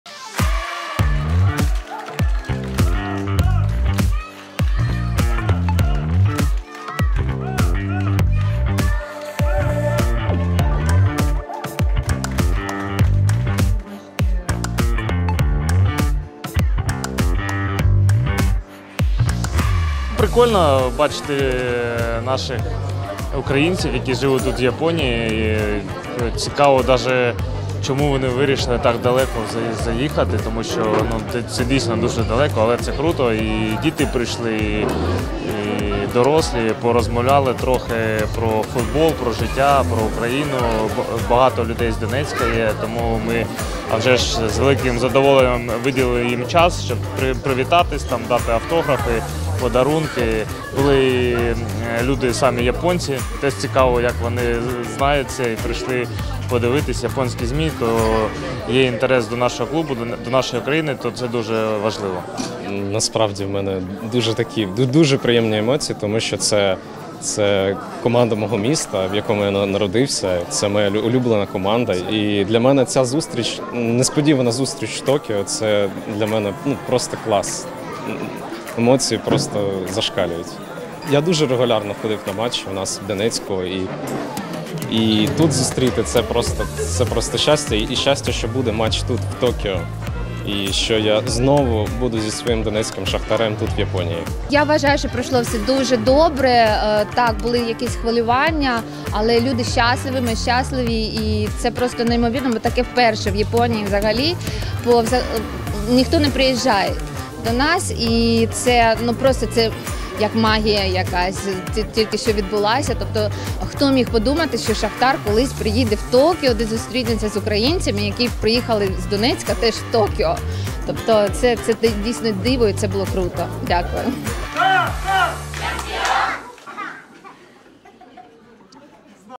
Шахтар зустрівся з українськими переселенцями у Японії
16 грудня в готелі Hilton Tokyo відбулася зустріч гравців «Шахтаря» з українцями, які проживають у Японії. Захід було організовано Центром підтримки українців у Токіо за сприяння Посольства України в Японії.

У гості до «оранжево-чорних» приїхали близько 50 українських переселенців, у тому числі з Донбасу, серед яких було багато дітей. Зі співвітчизниками зустрілися футболісти команди Тарас Степаненко, Микола Матвієнко, Олександр Зубков та Валерій Бондар. Говорили про війну та футбол, багато згадували часи, коли «гірники» грали в Донецьку. Представники української спільноти відзначили, що дуже раді бачити команду в Токіо, адже «Шахтар» є першим українським клубом, який відвідав Японію.

Наприкінці зустрічі гравці роздали автографи й зробили фото з усіма присутніми, а ще гості отримали пам’ятні подарунки від ФК «Шахтар». Своєю чергою співвітчизники побажали команді успіхів та перемог і запевнили, що підтримуватимуть «гірників» на трибунах стадіону у товариській грі в понеділок.

Нагадаємо, благодійний матч «Авіспа Фукуока» – «Шахтар» відбудеться 18 грудня на Японському національному стадіоні і почнеться о 12:00 за київським часом.